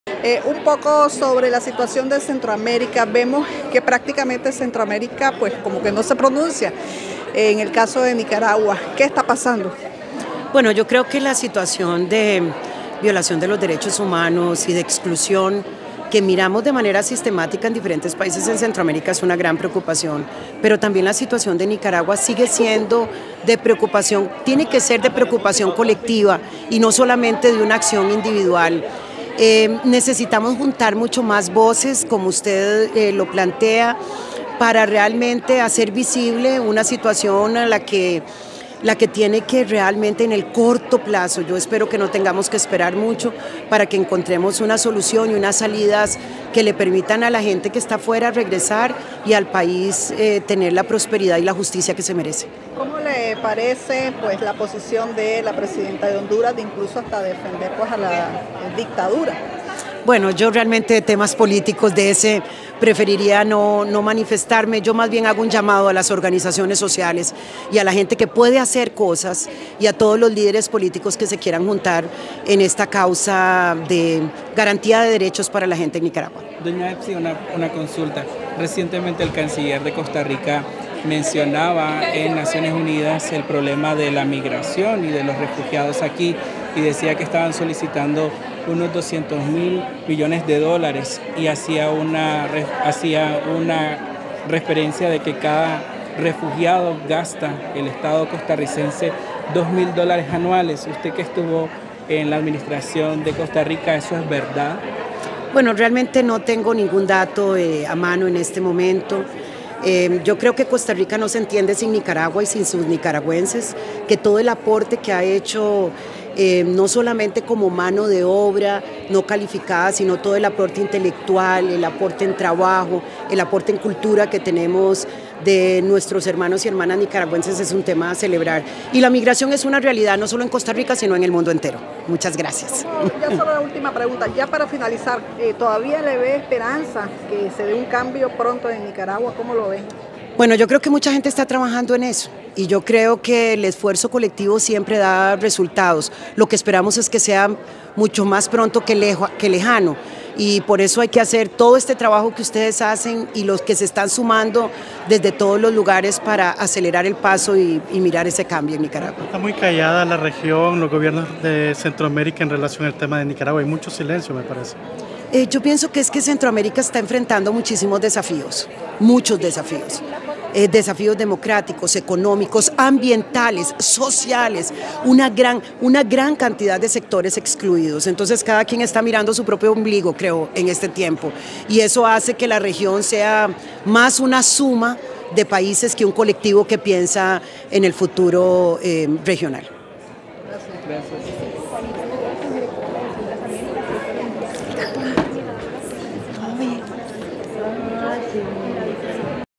Epsy Campbell explica por qué la crisis de Nicaragua no interesa al resto de Centroamérica
Una verdad irrefutable que sí reconoce la exvicepresidenta Epsy Campbell, quien partió por demandar el respeto a los derechos de los refugiados y emigrantes así como exhortó a no minimizar el valor agregado de sus vecinos.